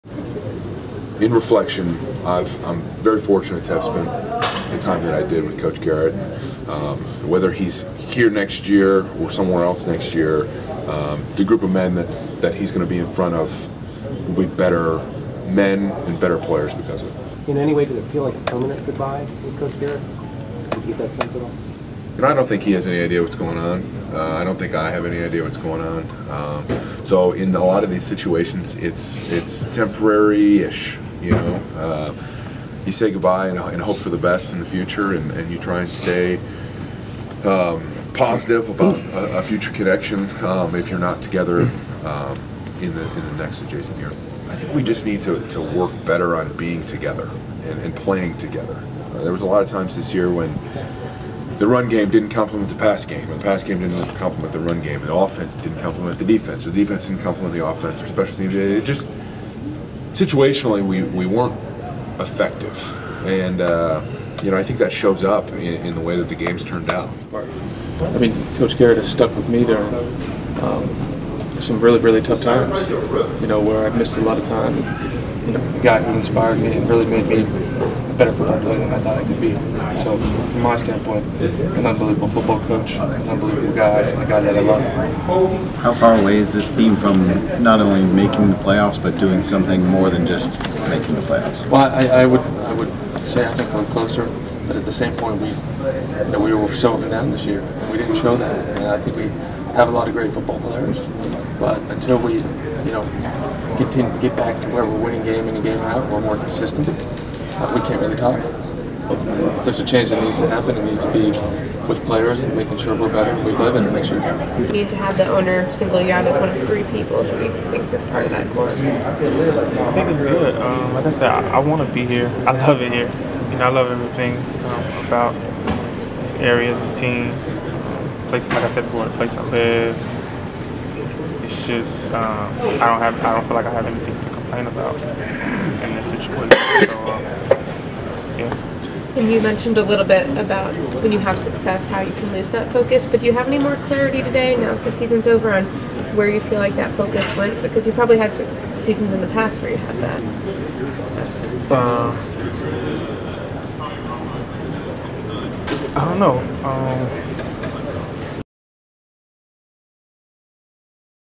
Dallas Cowboys Players Clean Out Lockers
Dallas Cowboys players Travis Frederick, Sean Lee and Amari Cooper talk to reporters after a team meeting at The Star on Dec. 30, 2019 in Frisco. The Cowboys season ended after failing to make it to playoffs. (Juan Figueroa/ The Dallas Morning News)